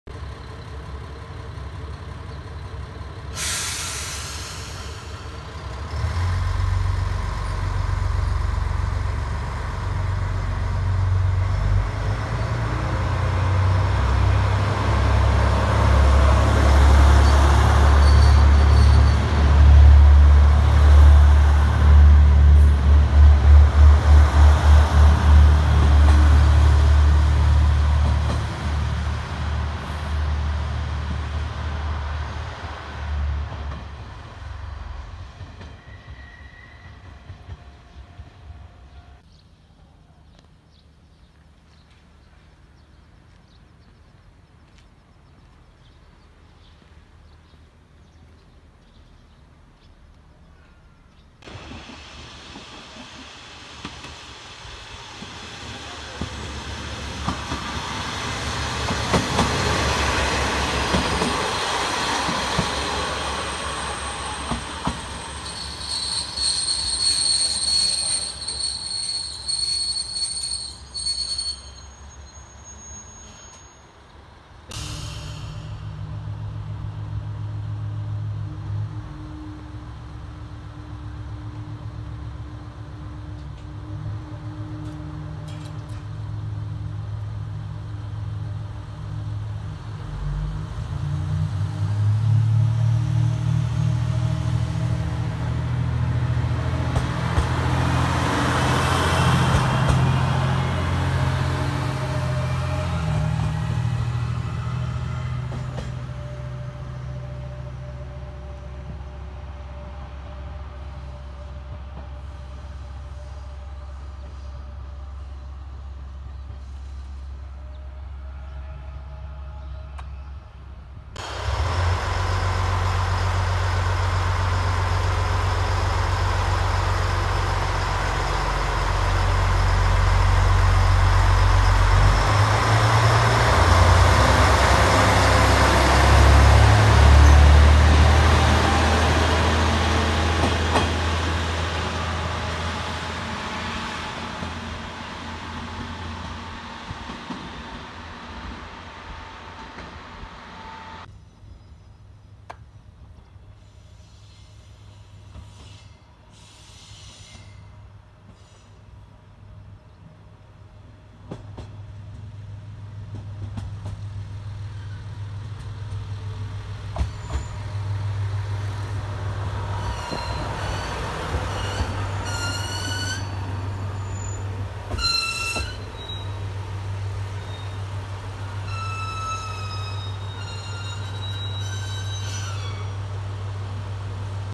後免駅で各駅停車をゆるく撮り鉄
後免駅でJR四国の土讃線および土佐くろしお鉄道ごめんなはり線の9640形気動車をゆるく撮り鉄しました。JRは1000形気動車を撮っています。ごめんなはり線は、阪神タイガースラッピング車両がきました。また、宝くじ号も来て、撮影後にはこれに乗車しています。